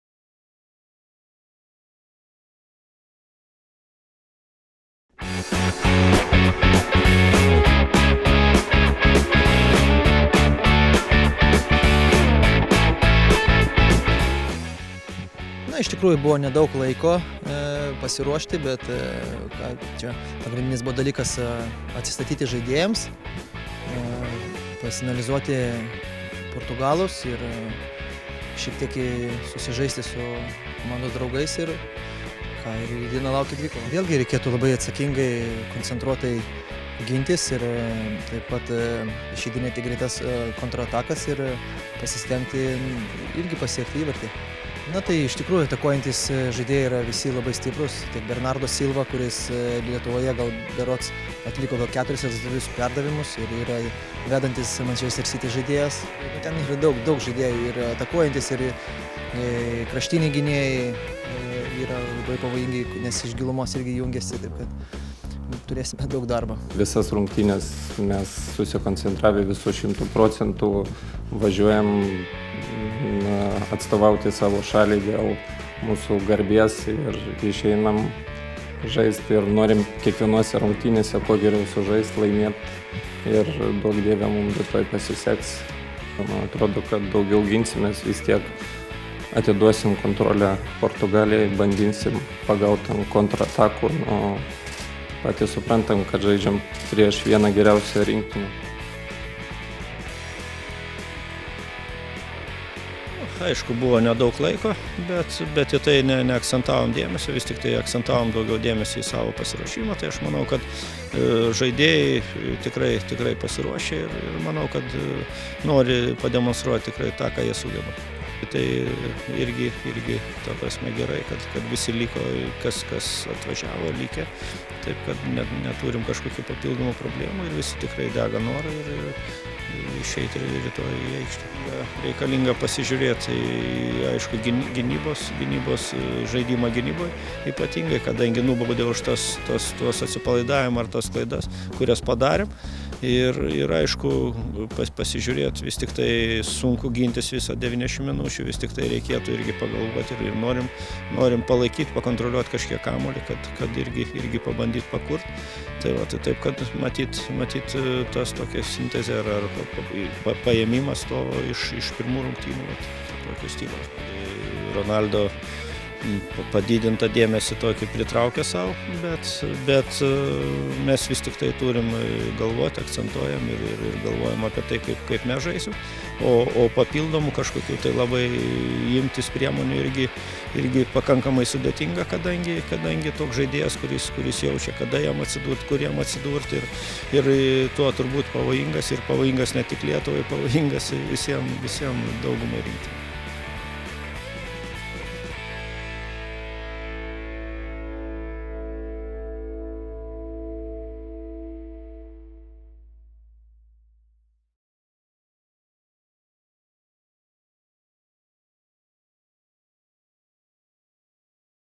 Lietuvos rinktinė pasiruošusi kovai su portugalais
🇱🇹 rinktinė jau šiandien stos į kovą su portugalais.

🎙 Žinome kur atvažiavome, ir kad jie žais pirmu numeriu. Reikės drausmingai gintis ir laukti savo šanso, - po paskutinės treniruotės sakė Saulius Mikoliūnas.

🎞 vaizdo reportaže taip pat kapitono Fiodoro Černycho ir trenerio Valdo Urbono mintys.
📺 transliacija 21:45 val. LRT